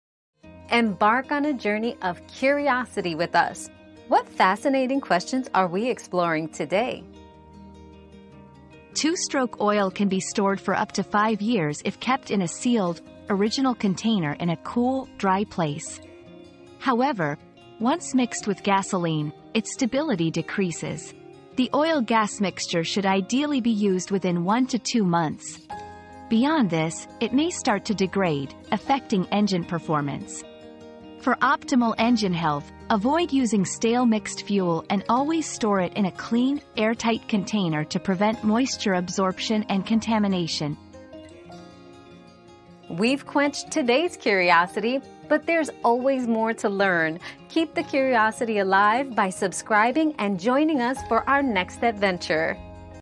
How long can 2 stroke oil sit?
How Long Can 2 Stroke Oil Sit? Important Tips for Storage and Usage 👉 2 Stroke Oil Storage Tips 👉 Learn how long 2 stroke oil can be stored and how to maintain its stability for optimal engine performance. Follow these tips to ensure your oil-gas mixture stays fresh and your engine runs smoothly.

Laura S. Harris (2024, February 21.) How long can 2 stroke oil sit?
    🌐 AskAbout.video